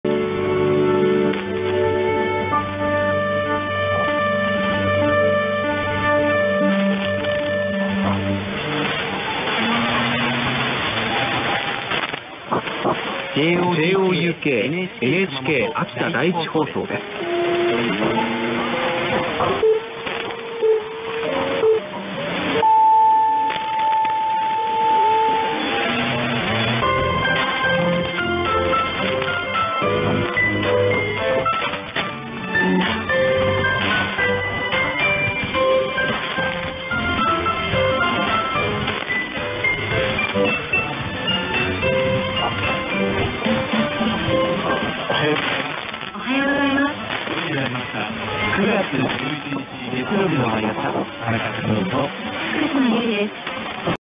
NHK局名アナウンス1503KHz NHK Kumamoto R1 JOGK Aso 2023 /09 /11 04: 59 JST
NHK熊本 JOGK　阿蘇中継局（1KW）
NHK秋田との混信が有ります
AIRSPY HF+ Discovery
ANT:303WA-2 , ATU ELEMENT,YOULOOP,  TDF, MLA, ALAM
QTH:SAITAMA JAPAN